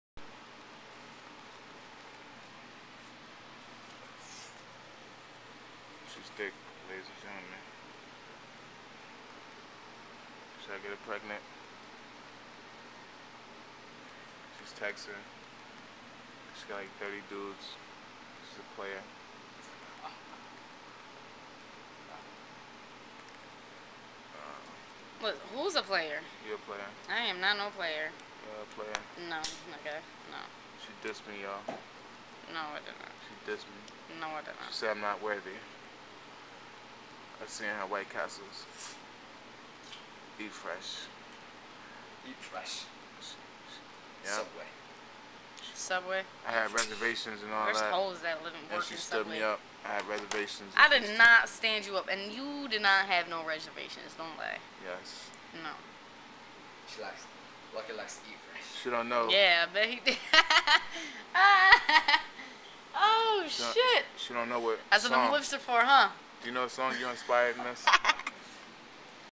Gutta From the Courts Baby Mother - "Tiffany Starkweather " in the swap shop before she got naked
Gutta From Hathorn Court Baby Mother - "Tiffany Starkweather " in the swap shop Doing sexual Favors!